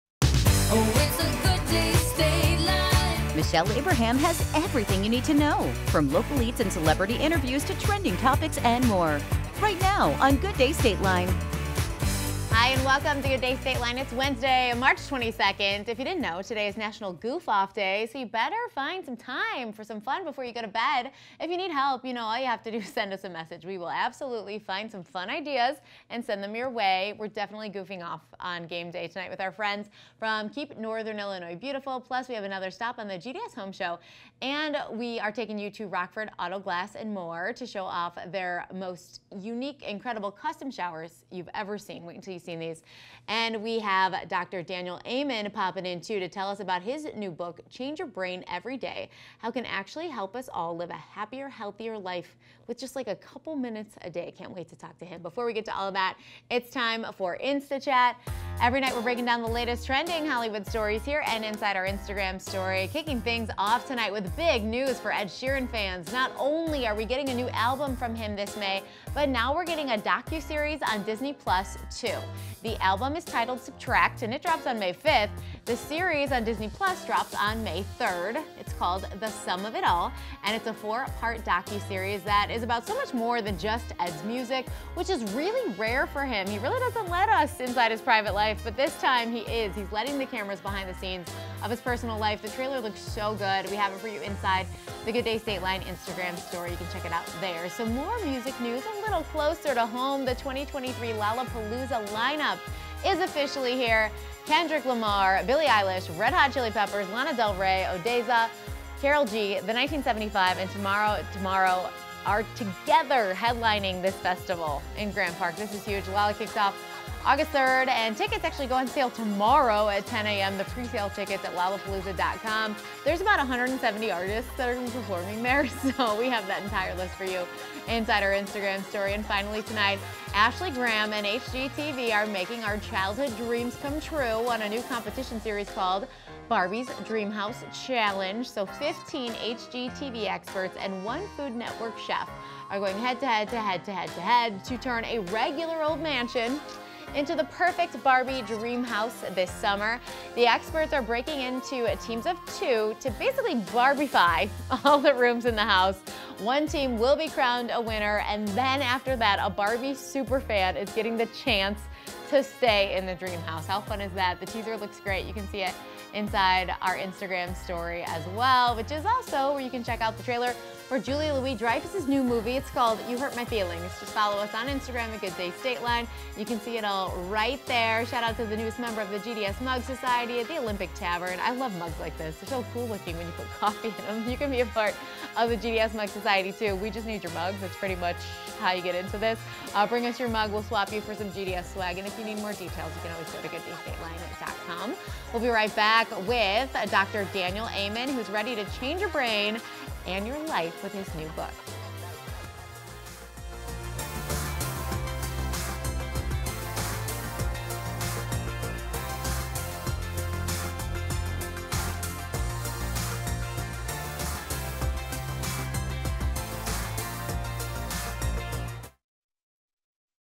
Ed Sheeran announces album and docuseries
Kicking things off tonight, Ed Sheeran announced the release date of his album and announced a four-part docuseries. The album is coming out May 5th and the docuseries drops May 3rd. Also tonight, the 2023 Lollapalooza lineup has been announced and Ashley Graham and HGTV are hosting the Barbie’s Dreamhouse Challenge. Check out these stories and more on our Instagram story at GoodDayStateline.